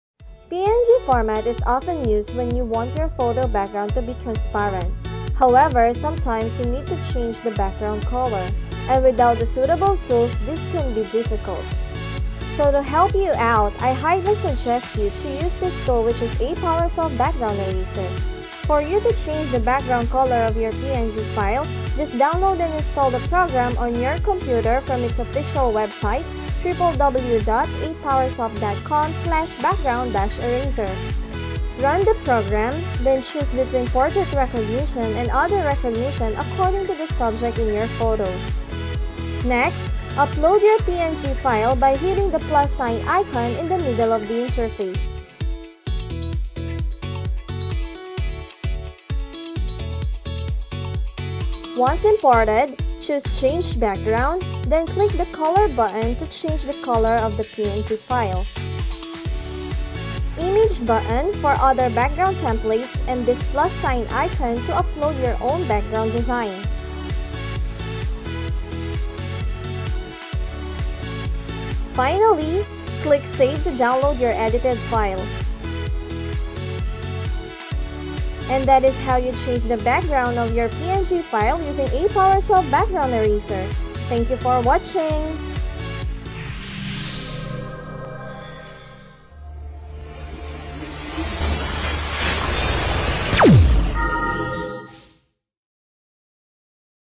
Best Software to Change PNG Background Color
Watch this video for you to discover the best tool to change your PNG background-color with just simple steps. And if you want a more informative one, read this article entitled "Best 5 Tools that can Change Color of PNG Image" in the link above.